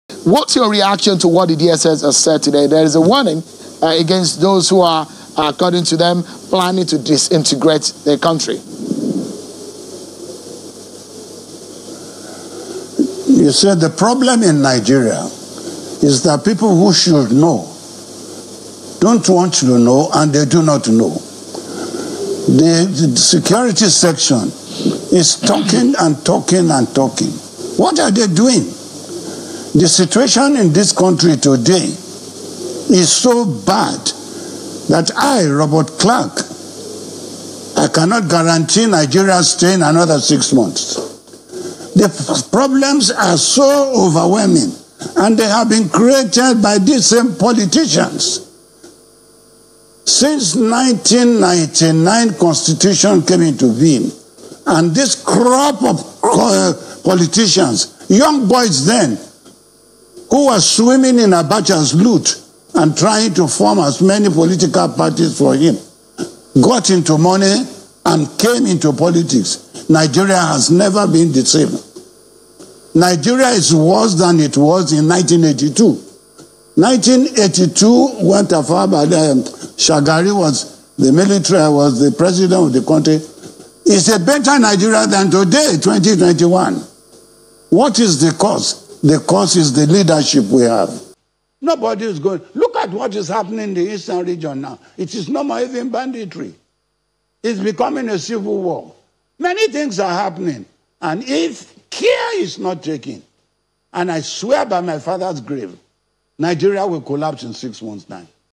Nigeria will collapse in 6 months time, I swear by my father’s grave -Buhari‘s ally, Clarke (SAN)
[VIDEO] Nigeria will collapse in 6 months time, I swear by my father’s grave -Buhari‘s ally, Robert Clarke (SAN)